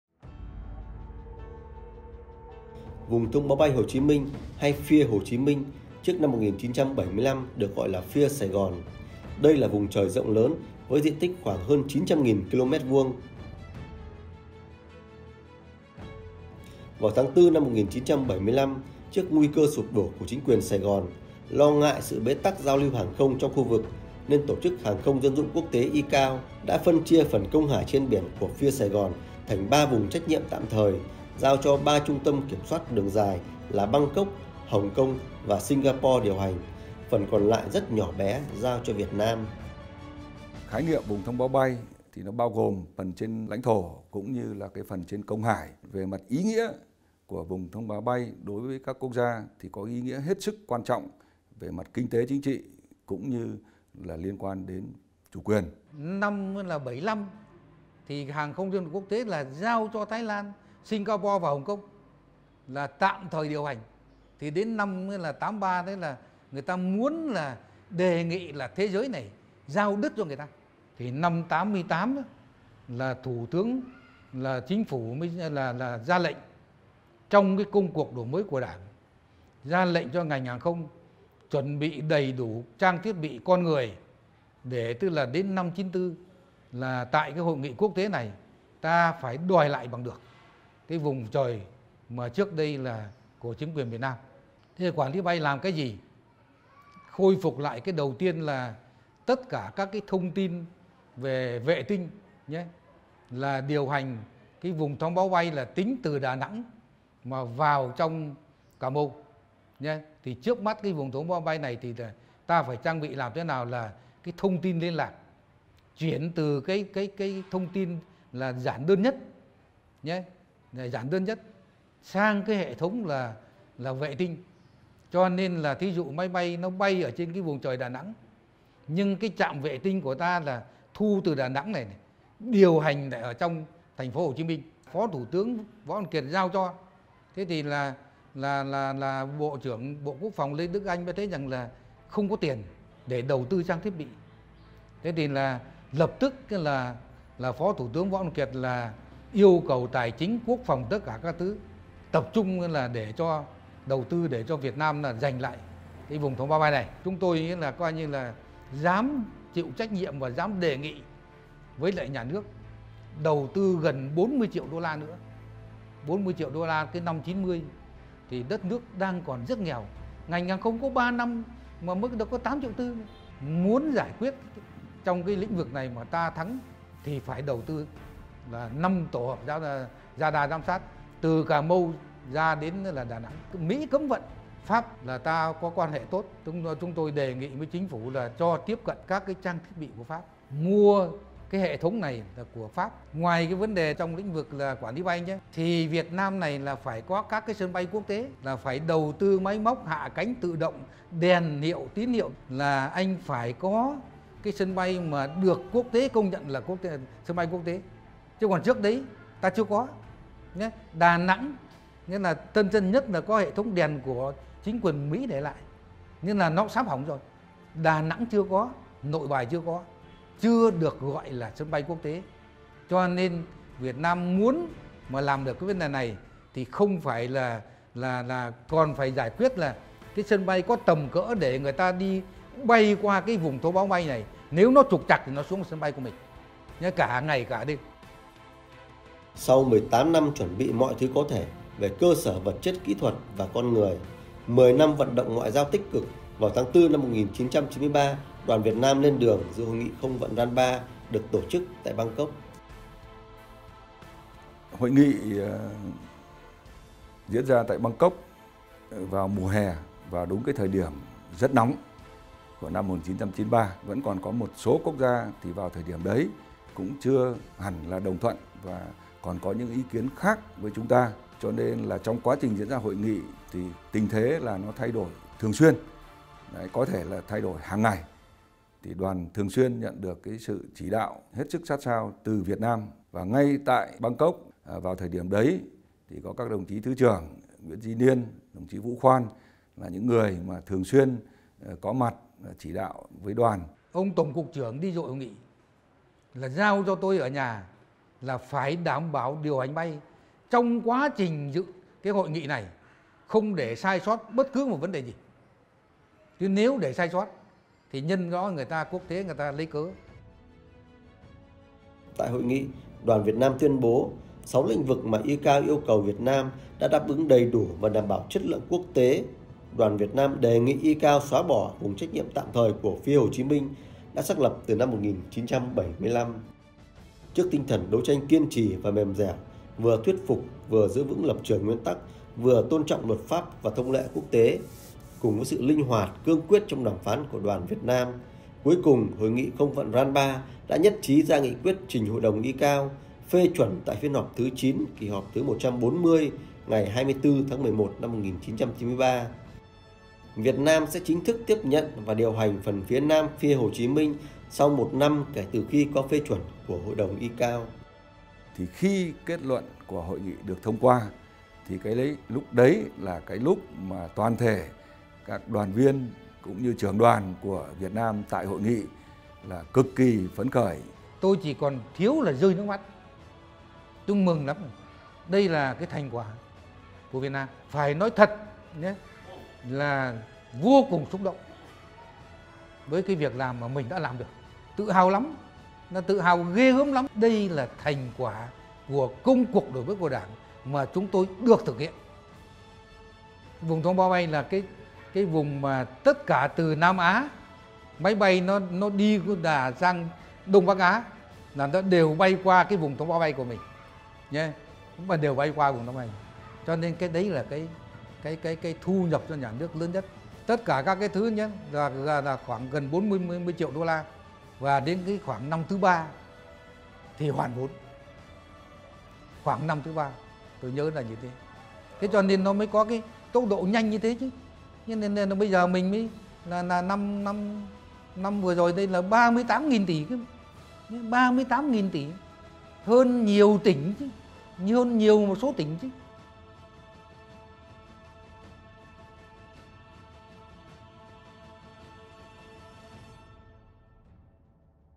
Cuộc đấu tranh giành lại vùng thông báo bay FIR Hồ Chí Minh [ GLS số 23 ]
Vùng thông báo bay Hồ Chí Minh hay FIR Hồ Chí Minh trước năm 1975 được gọi gọi là FIR Sài Gòn/ là vùng trời rộng lớn với diện tích khoảng hơn 900.000km2.

Vào tháng 4/1975, trước nguy cơ sụp đổ của chính quyền Sài Gòn, lo ngại trước bế tắc giao lưu hàng không trong khu vực/ nên Tổ chức Hàng không Dân dùng Quốc Tế ICAO/ đã phân chia phần công hải trên biển Đông của FIR Sài Gòn thành ba vùng trách nhiệm tạm thời/ giao cho 3 trung tâm kiểm soát đường dài là Bangkok, Hong Kong và Singapore điều hành; phần còn lại rất nhỏ giao cho Việt Nam.